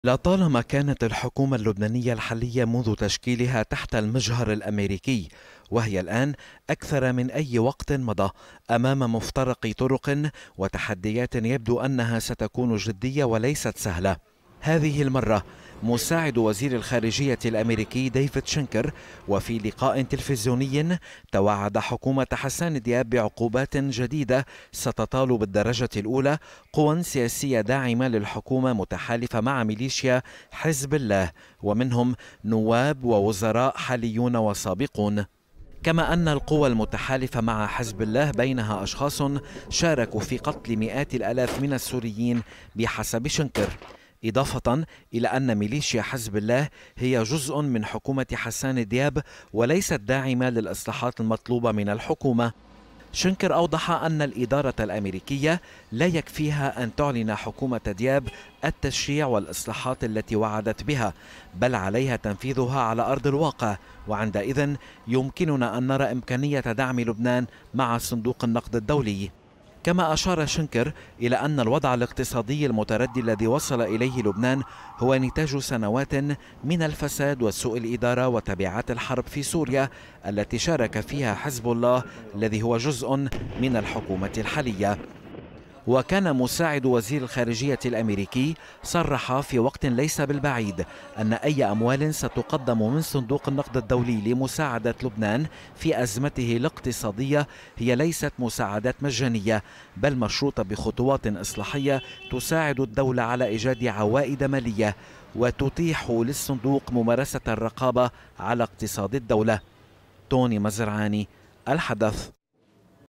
لبنان | القوى السياسية المتحالفة مع ميليشيا حزب الله في دائرة العقوبات الأميركية
قبل الشروط الداخلية كانت هناك شروط خارجية حيث قال مساعد وزير الخارجية الاميركية لشؤون الشرق الأوسط ديفيد_شنكر في حوار تلفزيوني إن الإدارة الأميركية تتطلع إلى فرض عقوبات على قوى سياسية داعمة لحكومة رئيس الوزراء اللبناني حسان_دياب وهي قوى متحالفة مع ميليشيا